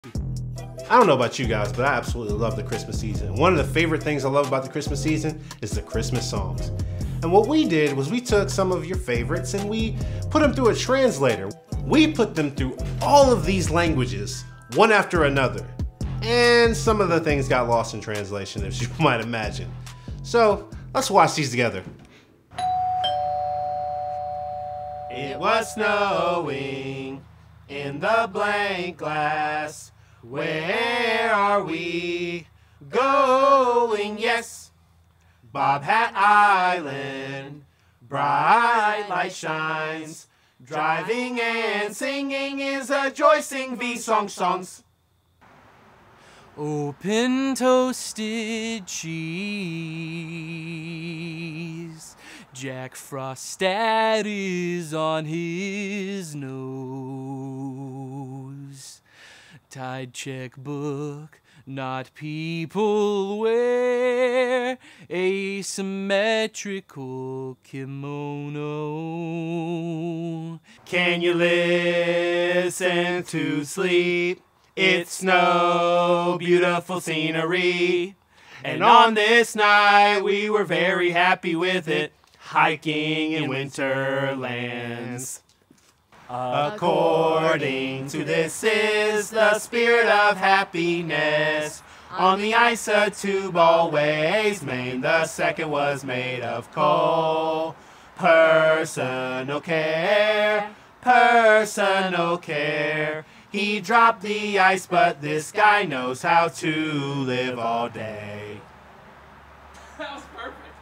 Christmas Remixed
Everyone all over the world loves Christmas songs. So we translated some of your favorites into several different languages, and a bunch of things got lost in translation.